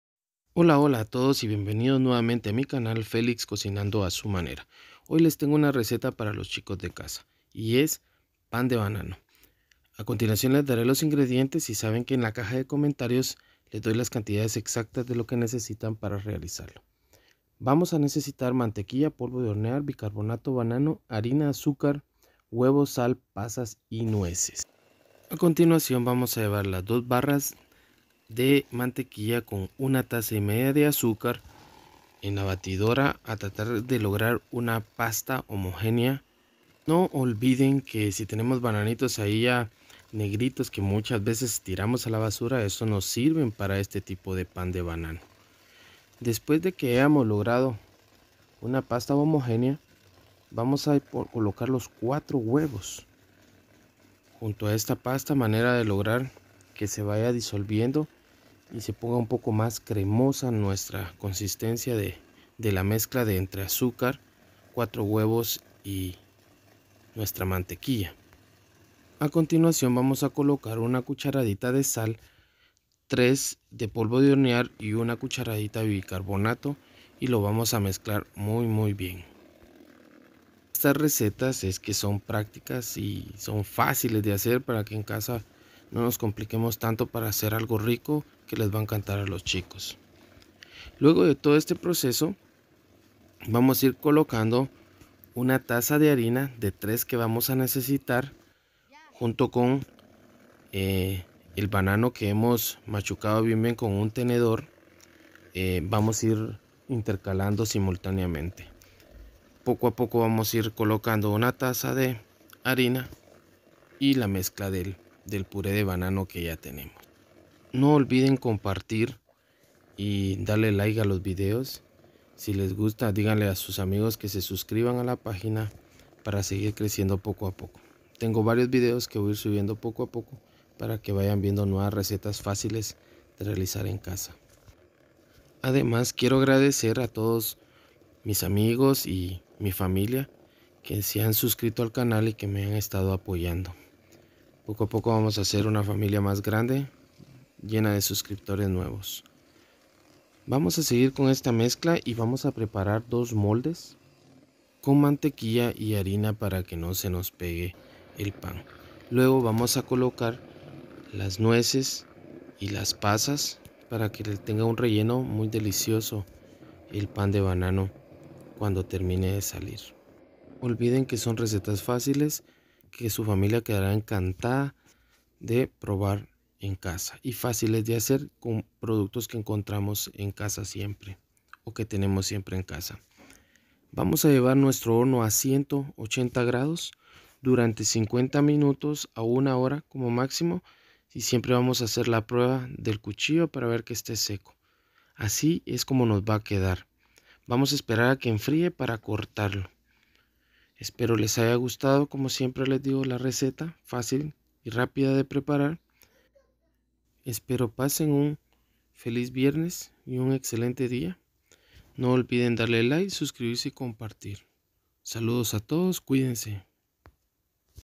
PAN DE BANANO FACIL Y RÁPIDO
06         bananos grandes machacados
03         tazas de harina
1 1/2    taza de azúcar
02          barras de mantequilla
04          huevos
01          cucharadita de bicarbonato
01          cucharadita de sal
03          cucharadita de polvo de hornear
pasas y nuez al gusto